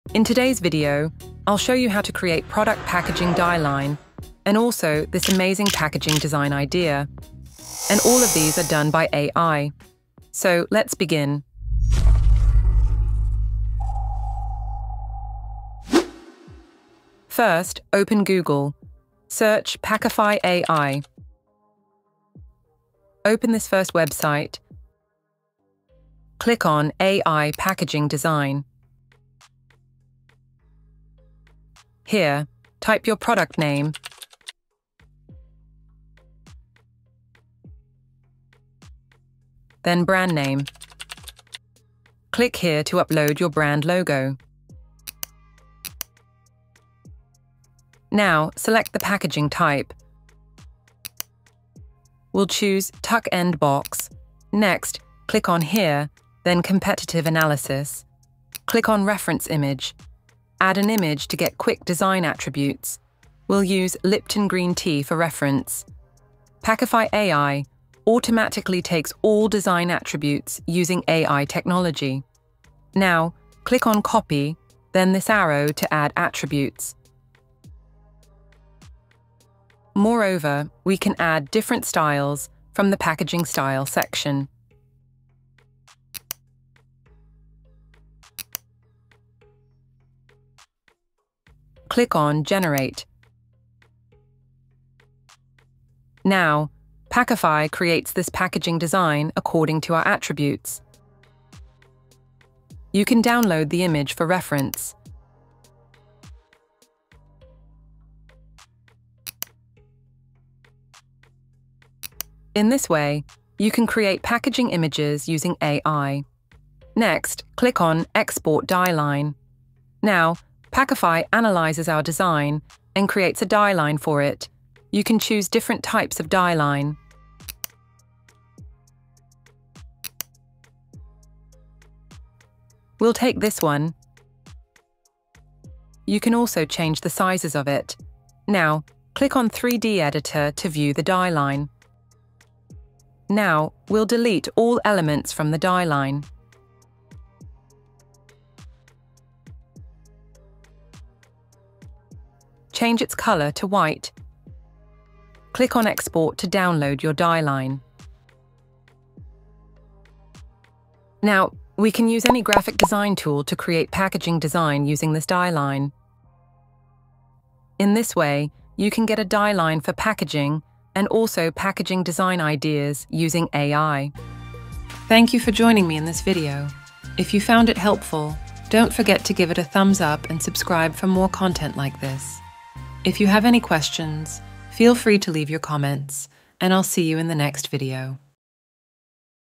Packaging & Dieline Creation Made Easy with Packify AI | Step-by-Step Tutorial
Want to design professional packaging without spending hours in complex software? 🚀 In this video, we’ll show you how to create stunning packaging designs and dielines using Packify AI**—perfect for brands, small businesses, and designers looking to save time.

✅ Learn how to generate custom dielinesusing AI
✅ Create packaging inspiration that’s print-ready
✅ Speed up your design workflow with AI tools

Whether you’re building a new product, running an eCommerce brand, or just exploring AI design tools, this tutorial will help you design smarter and faster.

💡 Tools Used: Packify AI (AI Packaging Design Tool)

Try it NOW for Free!

👉 Watch till the end for pro tips on making your packaging stand out!